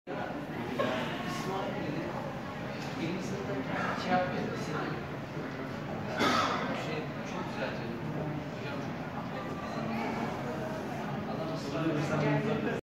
Çamlıca Kulesinden Ayasofya Camii
Çamlıca Kulesi'nden Ayasofya Cami'nin görüntüsü